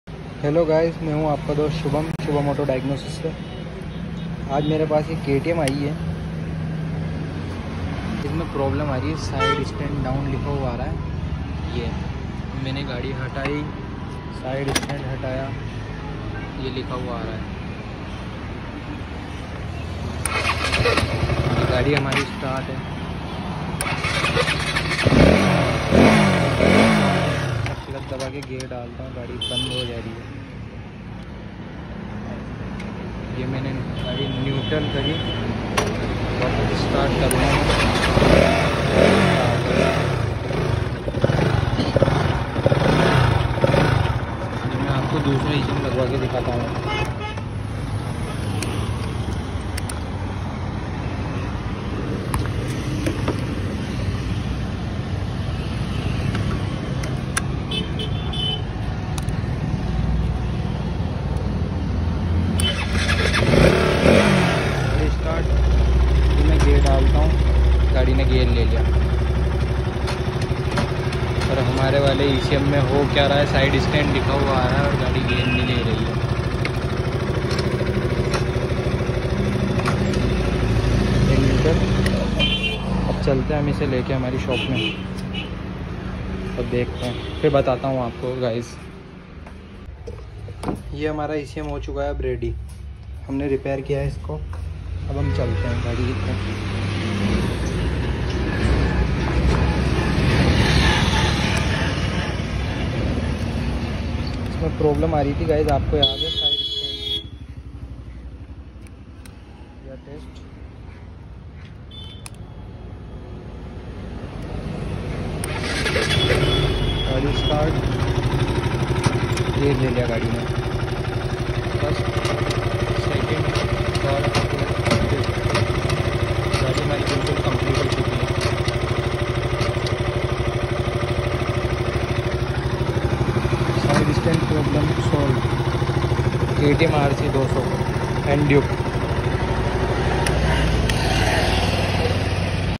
Side stand problem solved ktm rc 200 ￼
India main sab sa phala main na ya problam solve kari ha ktm k ecm main problam ati high coolant temperature￼ and side stand down problam solve now on shubham auto diagnosis kota Rajasthan